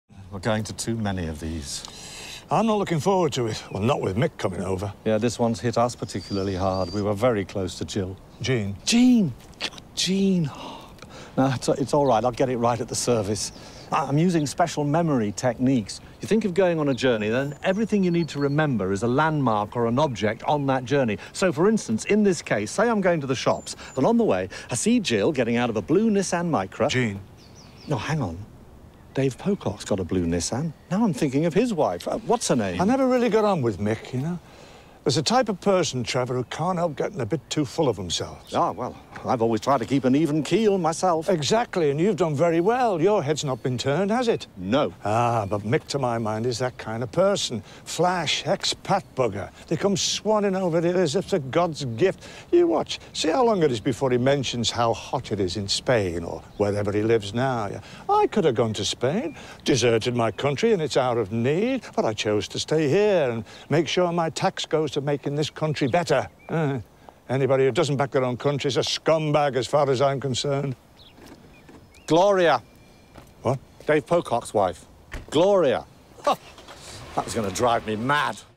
Memory Techniques For Old People - Boomers
John and Trevor discuss ways off remembering names, people and places somewhat unsuccessfully!